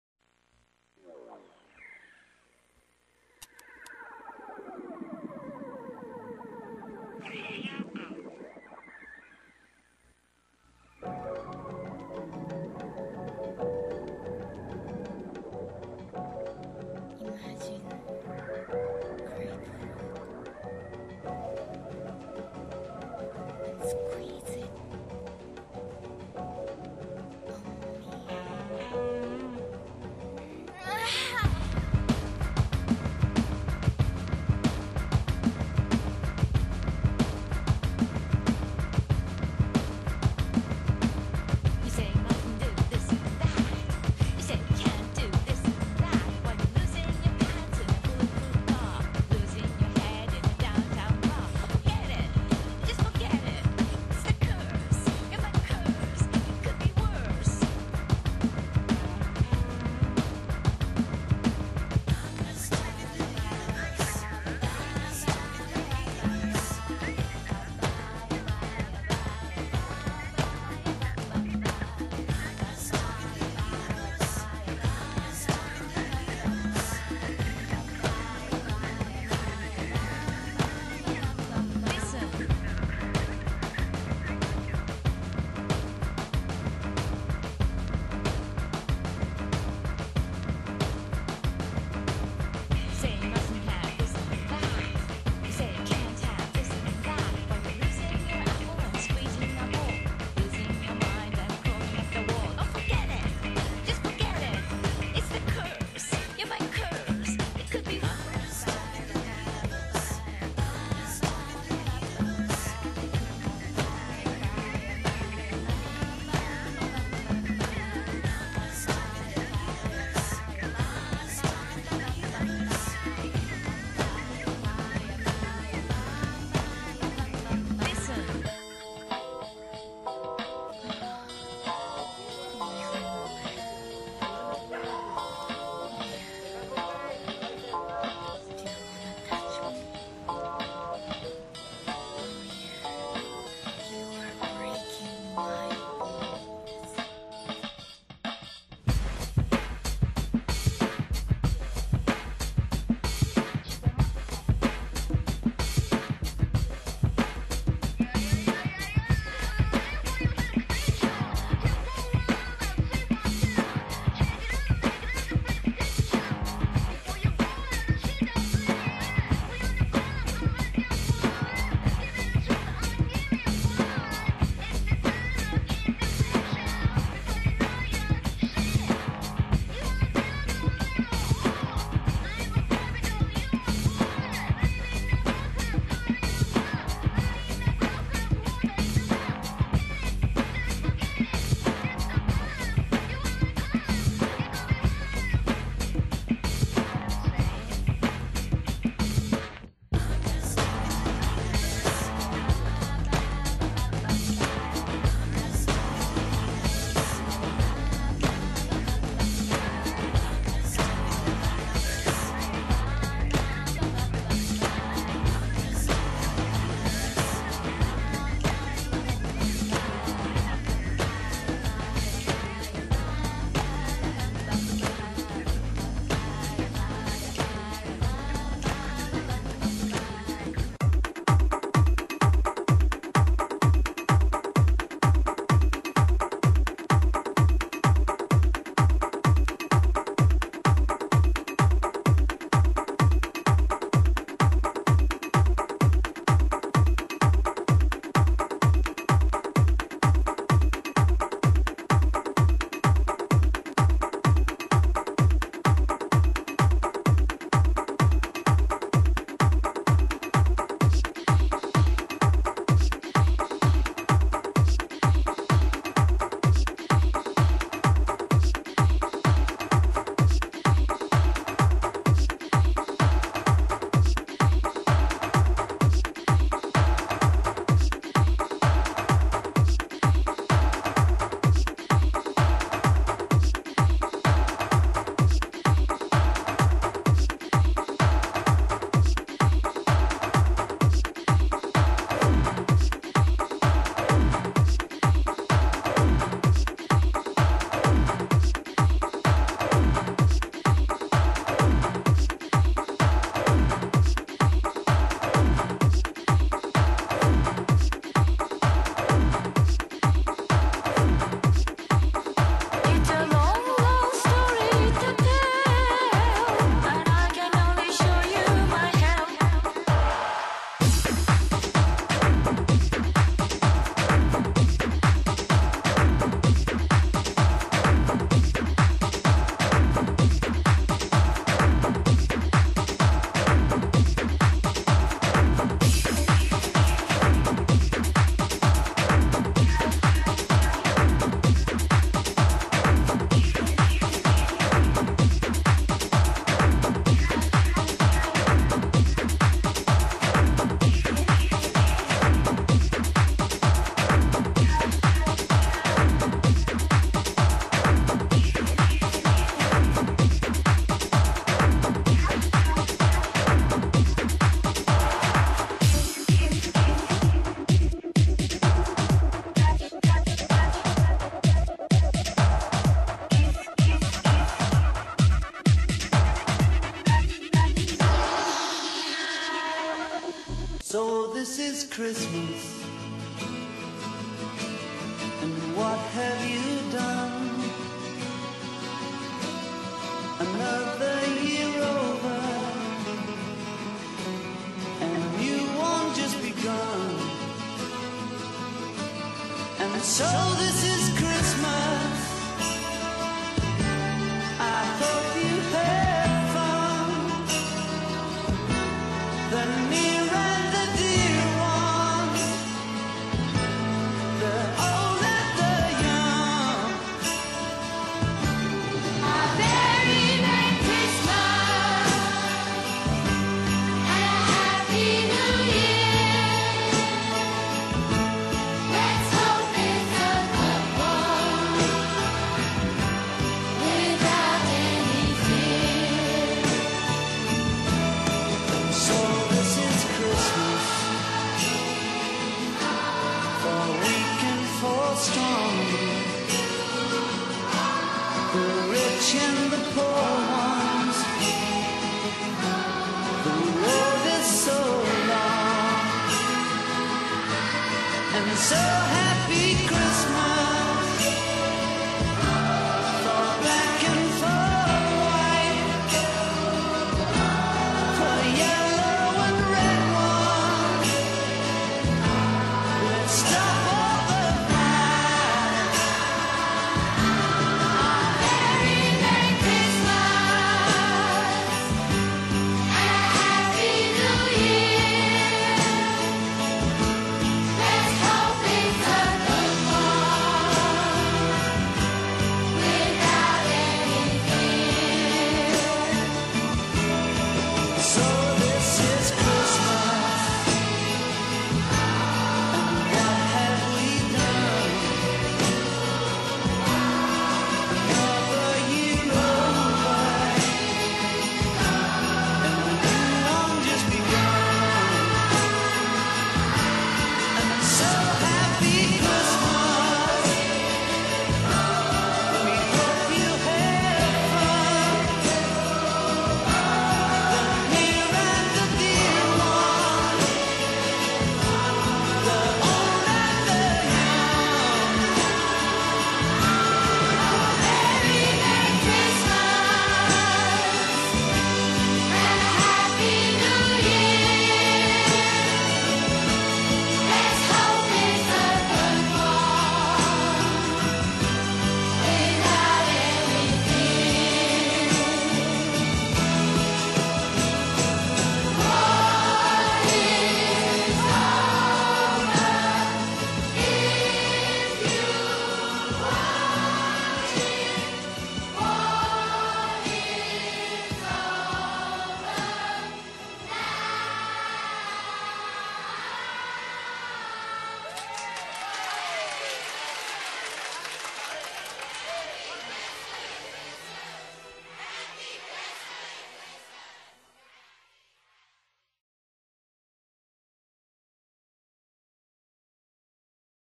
Yoko Ono Presentation
A report I am presenting in class at IADT